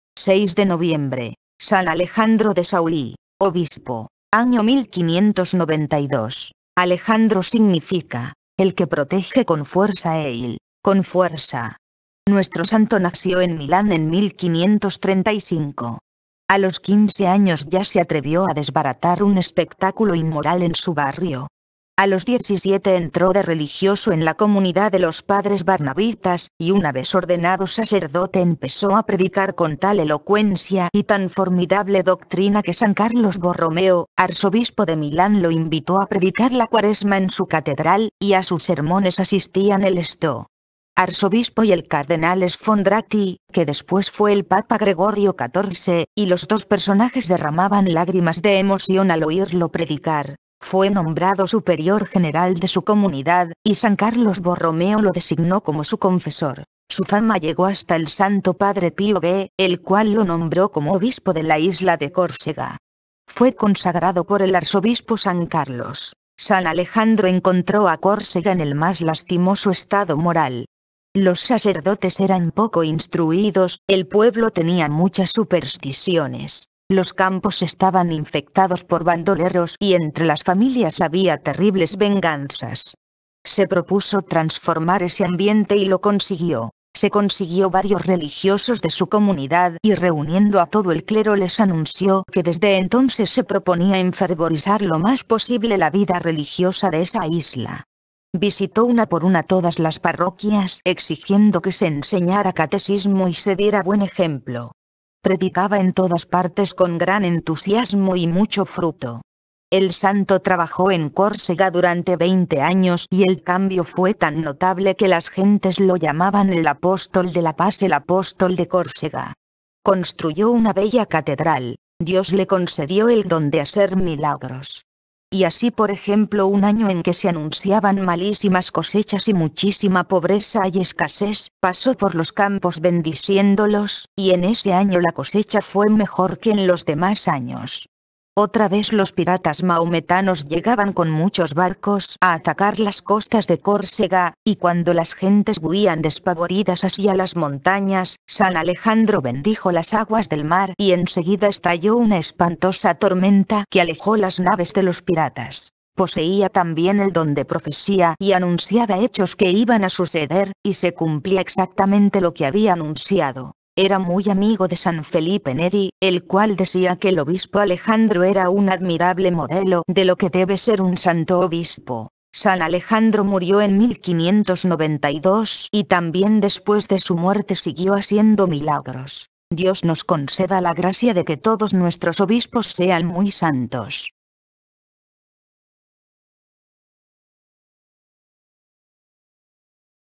San Alejandro de Sauli, 6 de Noviembre, Vidas Ejemplares
6 de Noviembre

 San Alejandro de Sauli
 Obispo
 Año 1592

Alejandro significa: "el que protege con fuerza" (Ale: con fuerza).